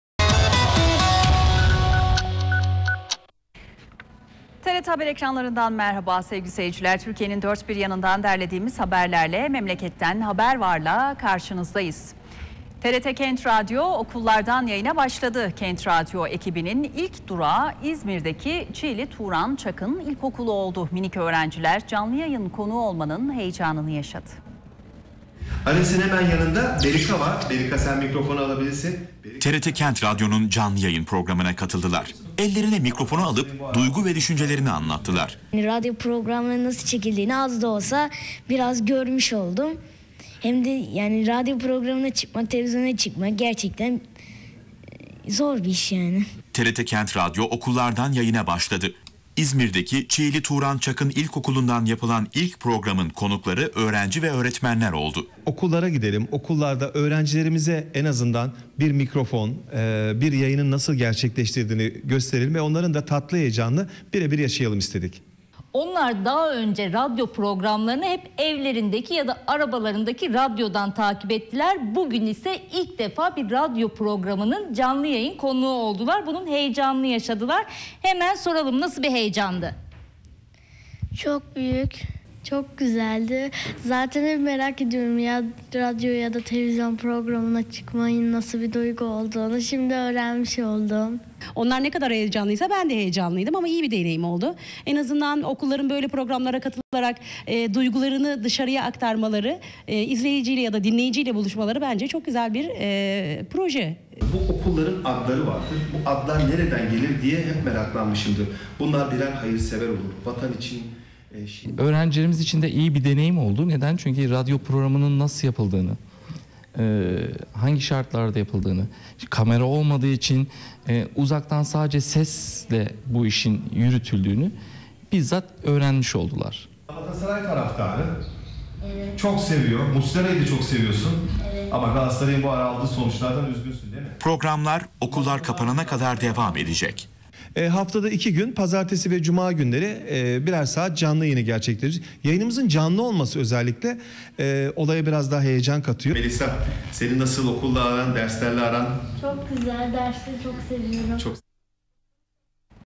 kent radyo
TRT Kent Radyo, okullardan yayına başladı. Kent Radyo ekibinin ilk durağı İzmir'deki Çiğli Turan Çakın İlkokulu oldu. Minik öğrenciler canlı yayın konuğu olmanın heyecanını yaşadı.